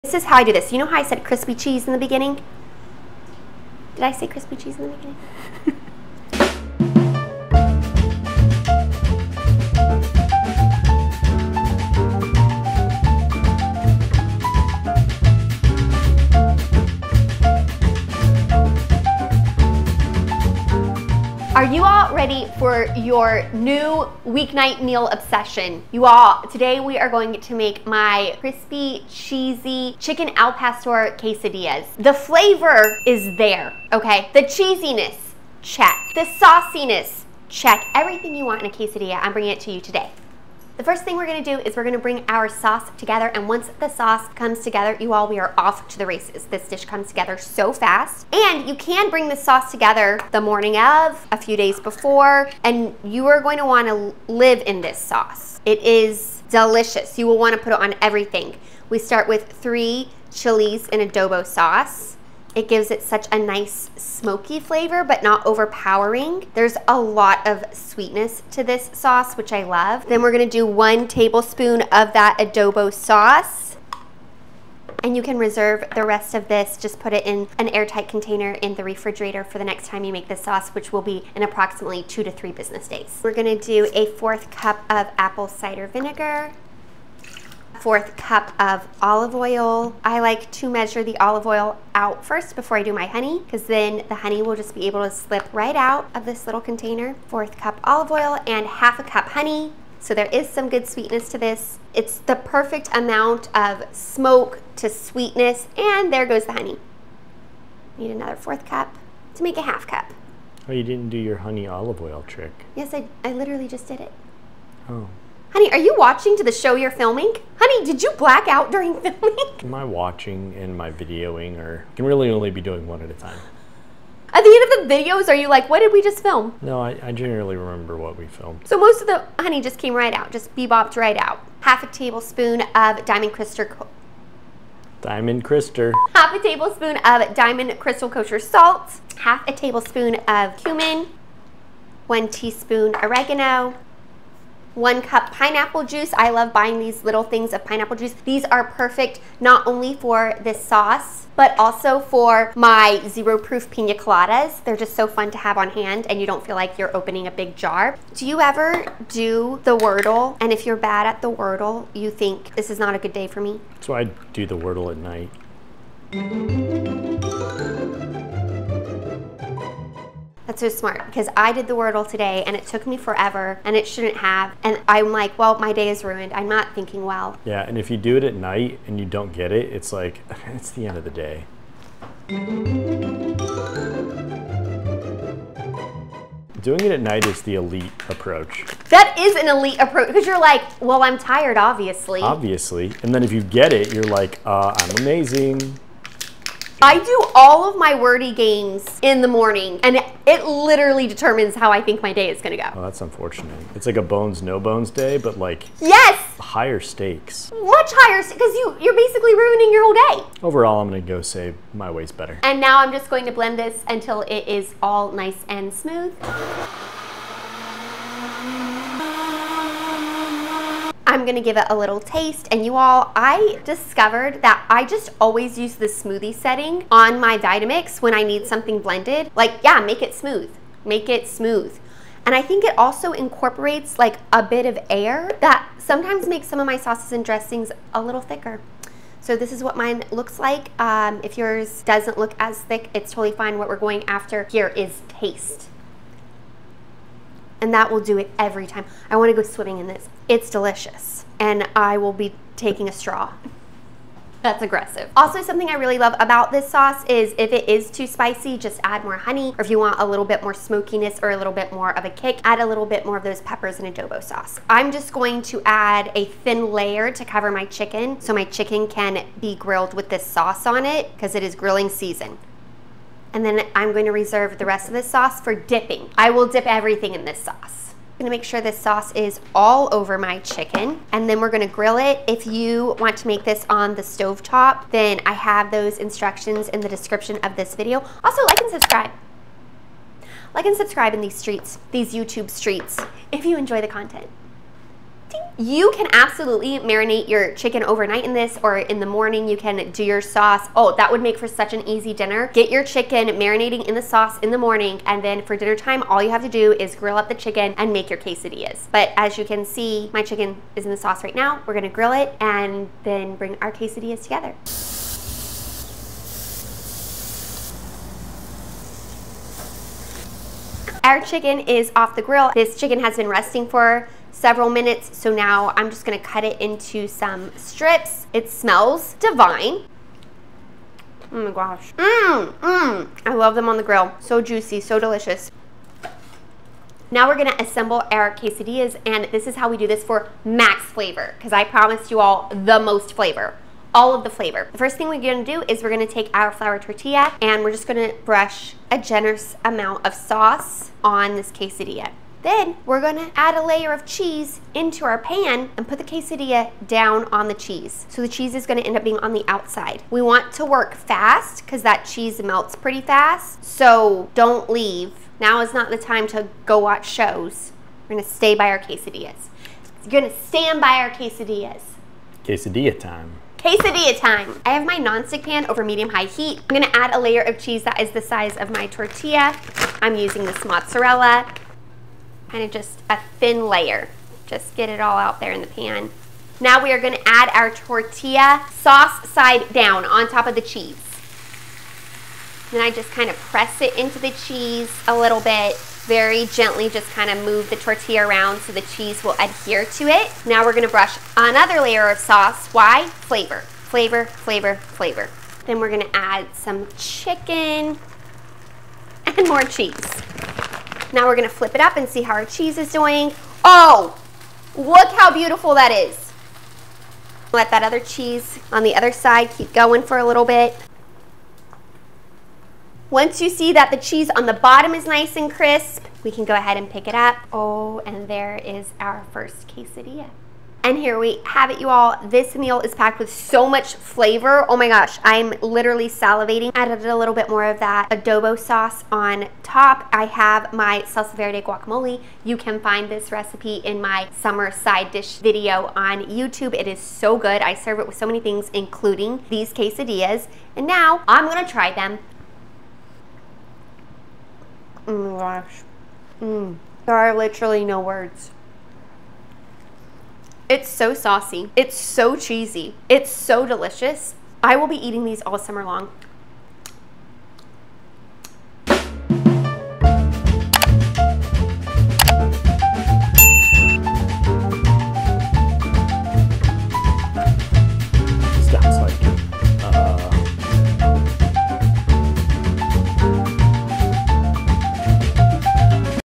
The Best Quesadillas: Cheesy al Pastor Quesadillas!
These quesadillas are saucy, cheesy and packed with flavor! Don't miss how to make these ahead of time and more importantly, Jeremy's take on Wordle 😅